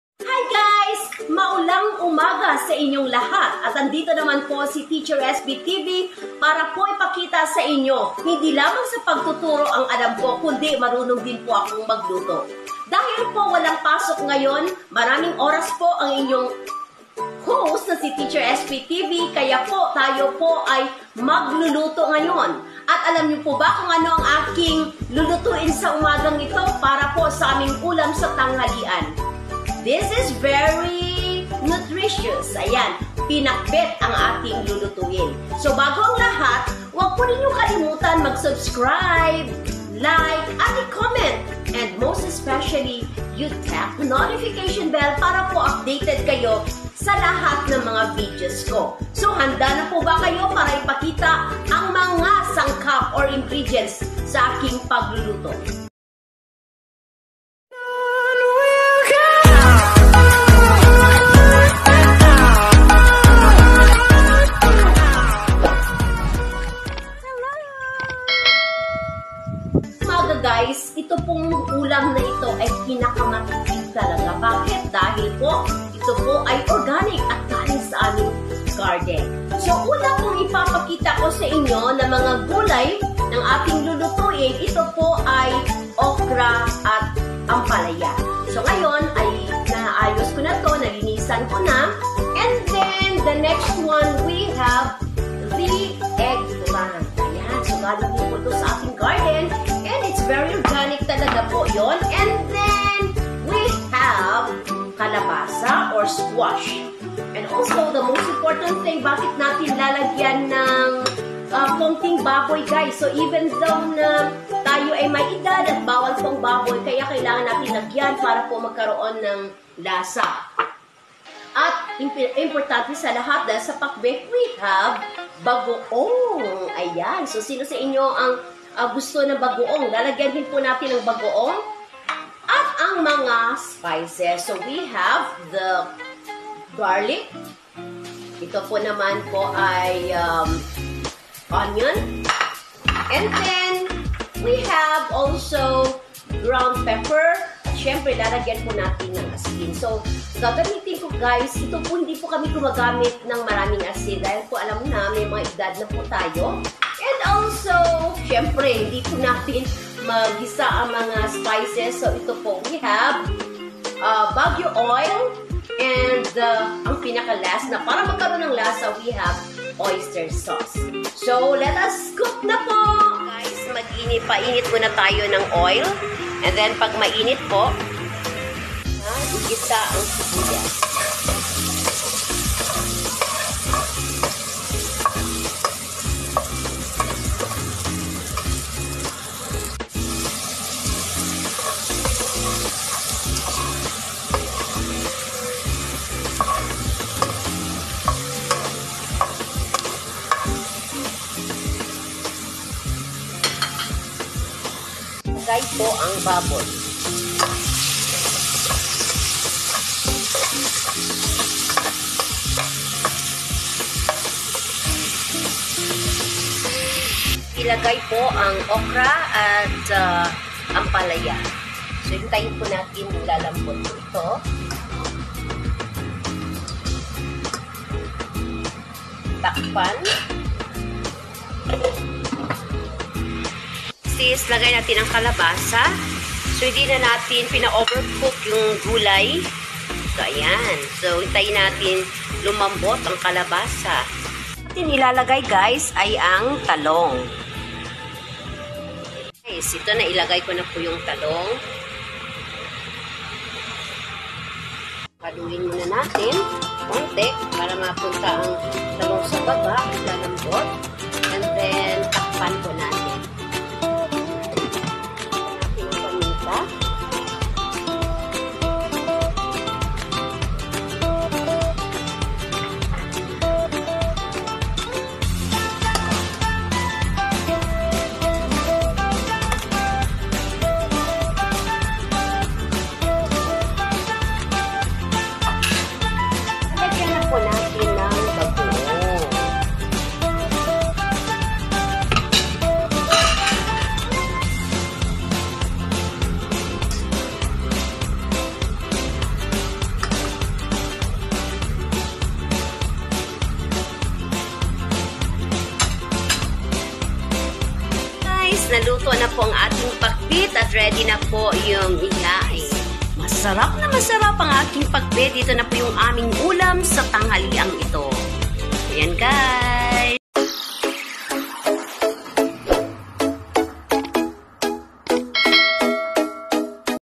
Pinakbet /Teacher Espie TV
I cook pinakbet and I.got all the vwgetablea from from our garden